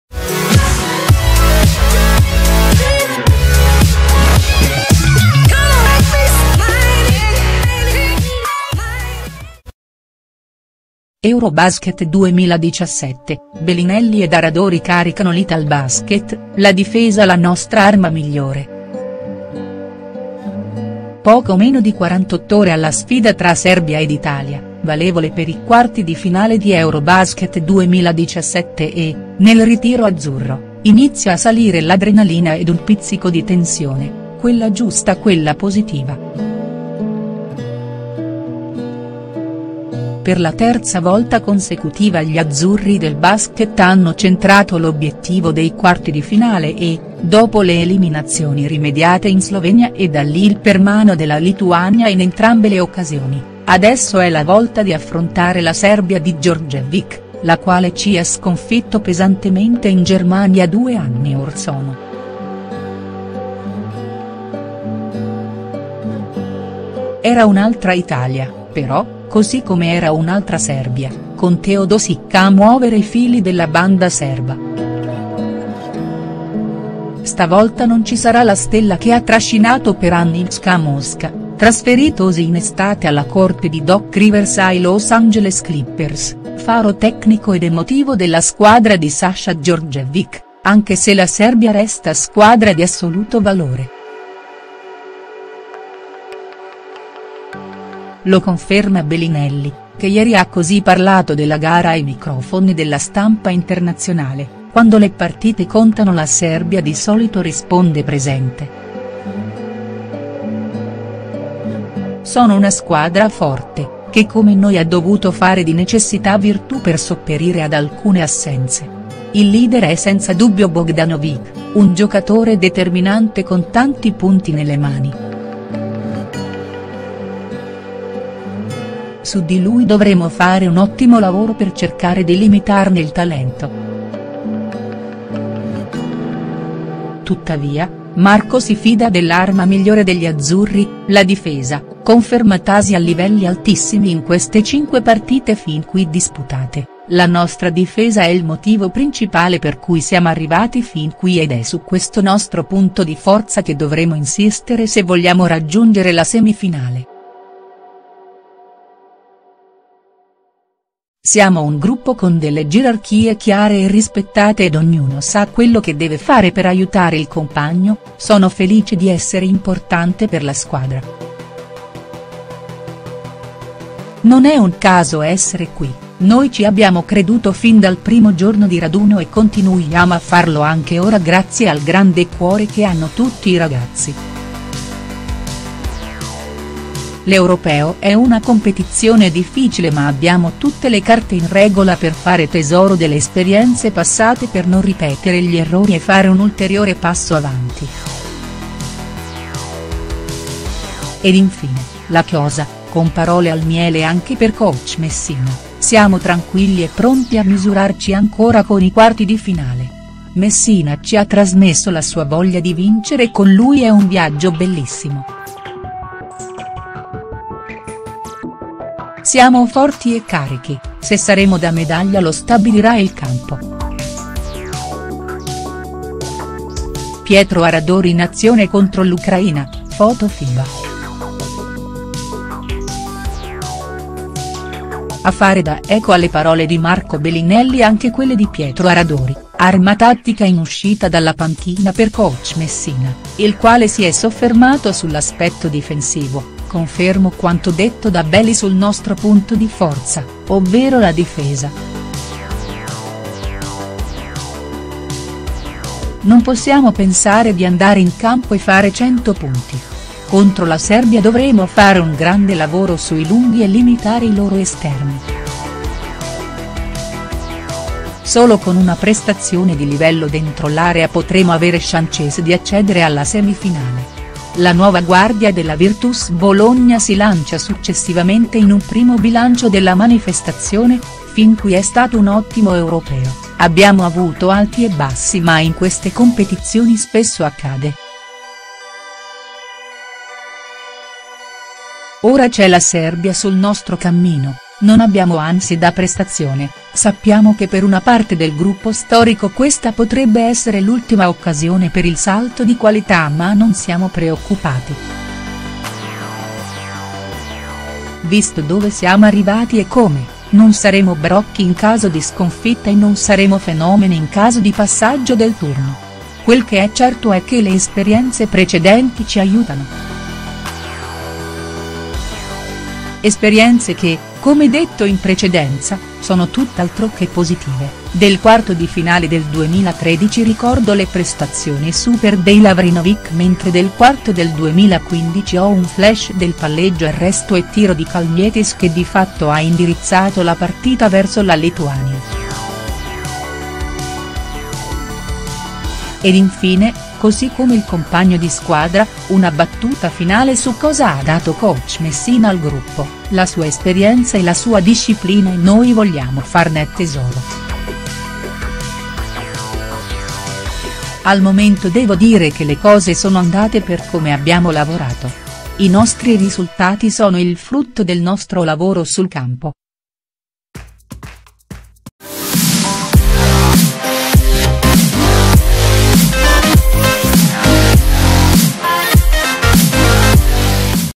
EuroBasket 2017 - Belinelli ed Aradori caricano l'ItalBasket: "La difesa la nostra arma migliore".HD
I due esterni azzurri, a due giorni dalla sfida contro la Serbia, hanno parlato ai media internazionali, anticipando in parte i temi della partita di domani sera.